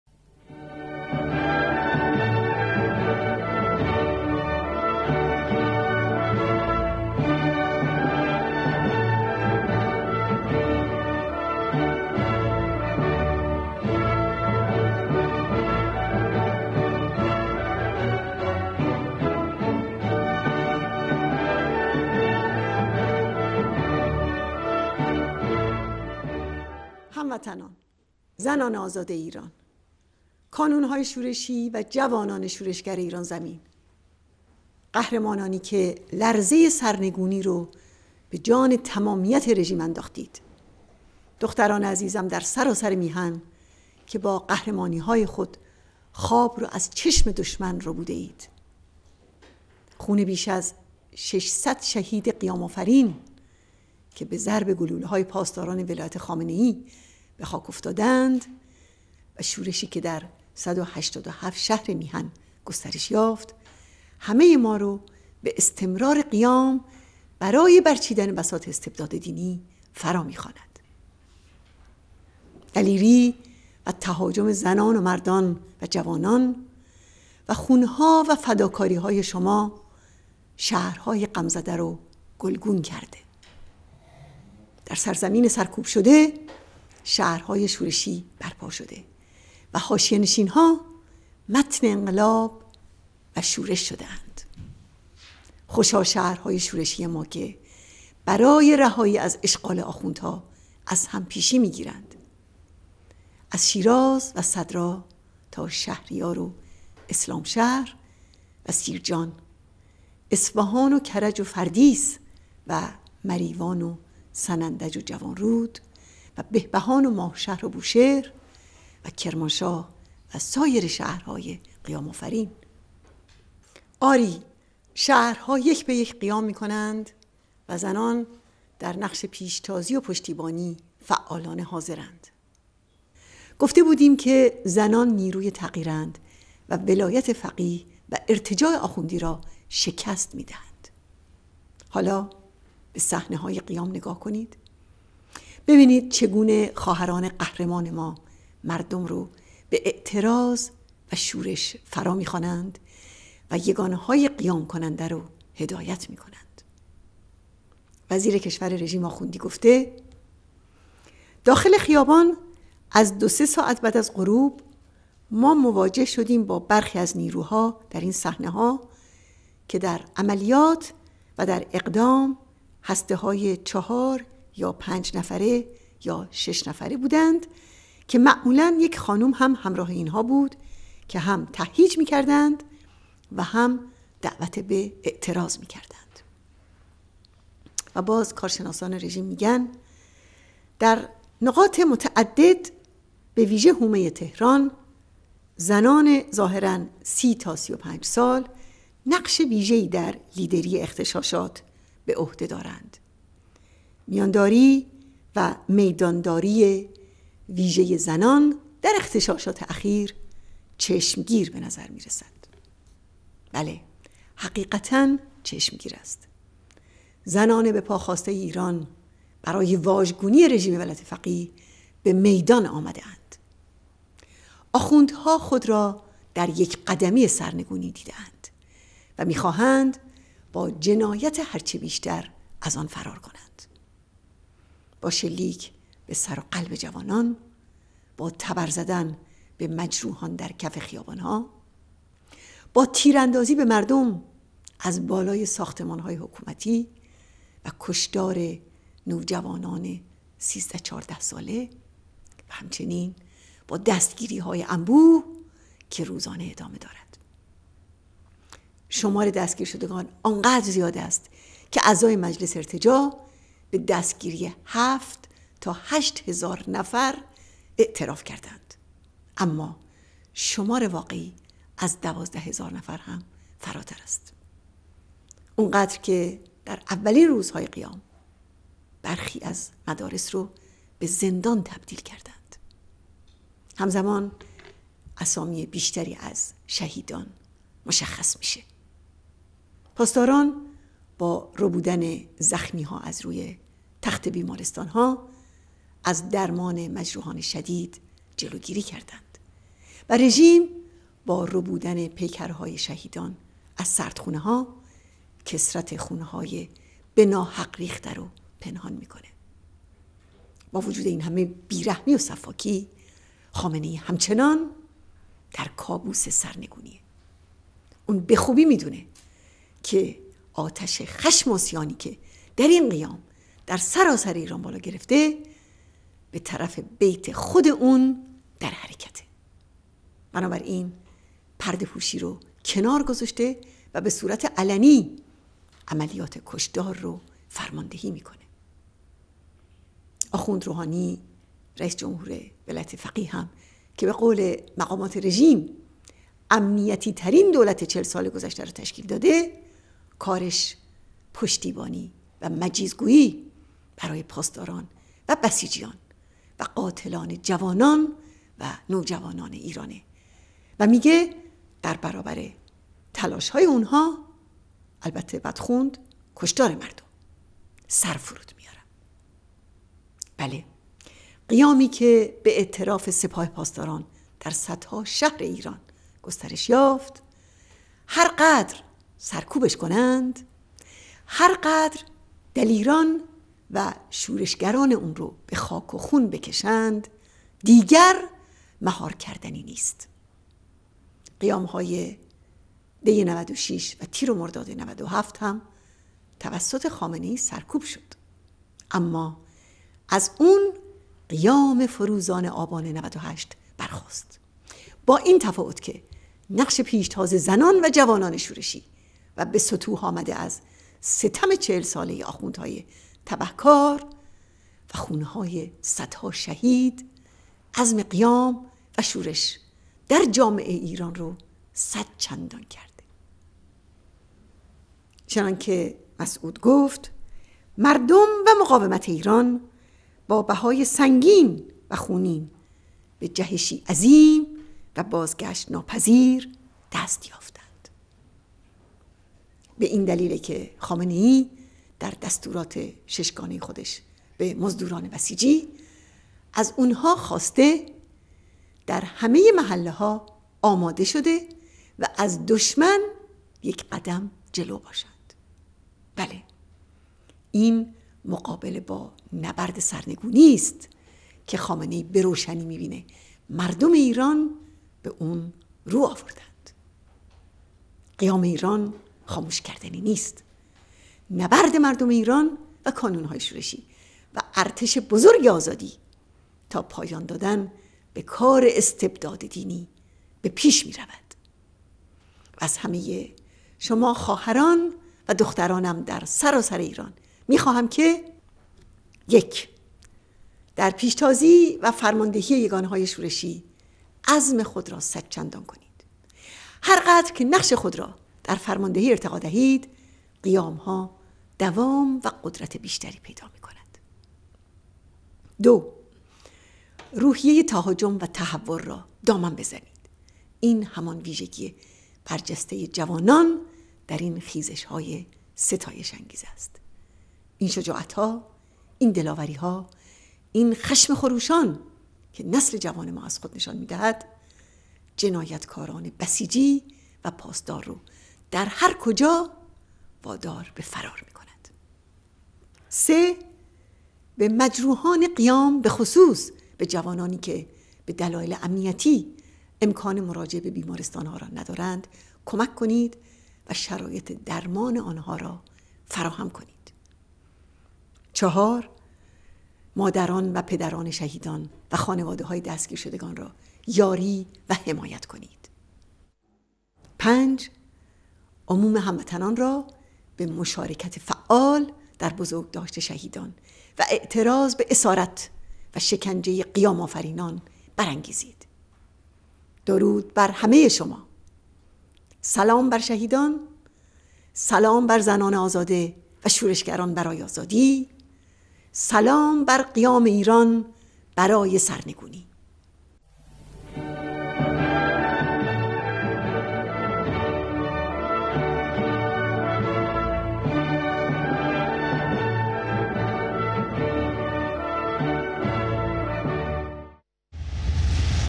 پیام رئیس جمهور برگزیده مقاومت خانم مریم رجوی- ۱۱ آذر ۱۳۹۸
زنان نیروی تغییراند و ولایت فقیه و ارتجاع آخوندی راشکست می‌دهند

هموطنان ، زنان آزاده ایران،
کانون‌های شورشی و جوانان شورشگر ایران زمین،
قهرمانانی که لرزه سرنگونی‌ را به جان تمامیت رژیم انداخته‌اید.
دختران عزيزم در سراسر ميهن كه با قهرمانيهاي خود خواب را از چشم دشمن ربوده‌ايد
خون بیش از ۶۰۰  شهید قیام آفرین که  به ضرب گلوله های پاسداران ولایت خامنه ای به خاک افتادند، و شورشی که در ۱۸۷شهر میهن گسترش یافت، همه ما را به استمرار قیام برای برچیدن بساط استبداد دینی فرا می‌خواند.
دلیری و تهاجم زنان و مردان و جوانان و خون‌ها و فداکاری‌های شما، شهرهای غمزده را گلگون کرده است. در سرزمین سرکوب شده شهرهای شورشی برپا شده و حاشیه‌نشین‌ها متن انقلاب و شورش گشته‌اند.
خوشا شهرهای شورشی ما که برای رهایی از اشغال آخوندها از هم پیشی می‌گیرند. از شیراز و صدرا تا شهریار و اسلام‌شهر و سیرجان، اصفهان و کرج و  فردیس و مریوان و سنندج و جوانرود و بهبهان و ماهشهر و بوشهر و کرمانشاه و سایر شهرهای قیام آفرین.
آری شهرها یک به یک قیام میکنند و زنان در نقش پیشتازی و پشتیبانی فعالانه حاضر‌‌اند.
گفته بودیم که زنان نیروی تغییراند و ولایت فقیه و ارتجاع آخوندی را شکست می‌دهند.
حالا به‌صحنه‌های قیام نگاه کنید ببینید چگونه خواهران قهرمان ما مردم را به‌اعتراض و شورش فرا می‌خوانند و یگان‌های قیام‌کننده را هدایت می‌کنند.
وزیر کشور رژیم آخوندی گفته است: «‌ داخل خیابان از دو سه ساعت بعد از غروب ما مواجه شدیم با برخی از نیروها در این صحنه‌ها که در عملیات و در اقدام هسته‌های ۴ یا ۵ نفره یا۶ نفره بودند که معمولا یک خانم هم همراه اینها بود که هم تهییج میکردند و هم دعوت به اعتراض میکردند».
و کارشناسان رژیم می‌گویند: « در نقاط متعدد به‌ویژه حومه تهران، زنان ظاهراً ۳۰ تا ۳۵‌سال، نقش ویژه‌ای در لیدری اغتشاشات به عهده دارند... میان‌داری و میدان‌داری ویژه زنان در اغتشاشات اخیر، چشمگیر به نظر می‌رسد».
بله، حقیقتاً چشمگیر است. زنان به‌پاخاسته ایران برای واژگونی رژیم ولایت فقیه به‌میدان آمده‌اند.
آخوندها خود را در یک قدمی سرنگونی دیده‌اند و می‌خواهند با جنایت هرچه بیشتر از آن فرار کنند. با شلیک به سر و قلب جوانان، با تبر زدن به‌مجروحان در کف خیابان‌ها، با تیراندازی به‌مردم از بالای ساختمان‌های حکومتی و کشتار نوجوانان سیزده، چهارده ساله، و همچنین با دستگیری‌های انبوه که هم‌چنان ادامه دارد.
شمار دستگیر شدگان آن قدر زیاد است که اعضای مجلس ارتجاع به دستگیری ‌هفت تا هشت هزار نفر اعتراف کرده‌اند اما شمار واقعی از ۱۲هزار نفر هم‌ فراتر است. آن‌قدر که در اولین روزهای قیام برخی از مدارس را به‌زندان تبدیل کردند.
هم‌زمان اسامی بیشتری از شهیدان مشخص میشود. پاسداران با ربودن زخمی‌ها از روی تخت
آخوند روحانی رئيس جمهور ولایت فقیه هم که به‌قول مقامات رژیم امنيتي ترين دولت ۴۰ سال گذشته را تشکیل داده، کارش پشتیبانی و مجیزگویی برای پاسداران و بسيجيان و قاتلان جوانان و نوجوانان ایران است و میگوید: «در برابر تلاش‌های‌ آن‌ها (البته باید خواند، کشتار مردم)...
 سر فرود می‌آورم».
بله، قیامی که به‌اعتراف سپاه پاسداران در صدها شهر ایران گسترش یافت، هر قدر سرکوب‌اش کنند، هر قدر دلیران و شورشگران‌ آن را به‌خاک و خون بکشند، دیگر مهار کردنی نیست . قیام‌های دی ۹۶ و تیر و مرداد ۹۷ هم توسط خامنه‌ای سرکوب شد،‌ اما از آن قیام فروزان آبان ۹۸ برخاست.
با این تفاوت که نقش پیشتار زنان و جوانان شورشی و به ستوه آمده از ستم چهل ساله آخوندهای تبهکار و خون‌های صدها شهید،  عزم قیام  و شورش در جامعه ایران را صد چندان کرده است.
چنان که مسعود گفت:‌ «مردم و مقاومت ایران با بهای سنگین و خونین به جهشی عظیم و بازگشت‌ناپذیر دست یافتند».
به‌این دلیل است که خامنه‌ای در دستورات شش گانه خود به‌مزدوران بسیجی از آن‌ها خواسته است در همه محله‌ها آماده و از دشمن یک قدم جلو باشند.
از همه شما خواهران و دخترانم در سراسر ایران می‌خواهم که:
۱ ـ  در پیشتازی و فرماندهی یگان‌های شورشی عزم خود را صد چندان کنید. هرقدر که نقش خود را در فرماندهی ارتقا دهید قیام‌ها دوام و قدرت بیشتری پیدا می‌کند.

۲ ـ‌ روحیه تهاجم و تهور را دامن بزنید این همان ویژگی برجسته جوانان در این خیزش‌های ستایش‌انگیز است. این شجاعت‌ها، این دلاوریها، این خشم خروشان  که نسل‌ جوان ما از خود نشان می‌دهد، جنایتکاران بسیجی و پاسدار را در هرکجا وادار به‌فرار می‌کند.
۳ ـ به‌مجروحان قیام به خصوص به جوانانی که به‌دلائل امنیتی امکان مراجعه به‌بیمارستان‌ها را ندارند، کمک کنید و شرائط درمان آن‌ها را فراهم کنید.
۴ ـ‌ مادران و پدران شهیدان و خانواده‌های دستگیرشدگان را یاری و حمایت کنید.
۵ ـ عموم هموطنان را به مشارکت فعال در بزرگداشت شهیدان و اعتراض به‌اسارت و شکنجه قیام‌‌آفرینان برانگیزید.
درود بر همة شما
سلام بر شهیدان
 سلام بر زنان آزاده و شورشگران براي آزادي
سلام بر قیام‌ ایران برای سرنگونی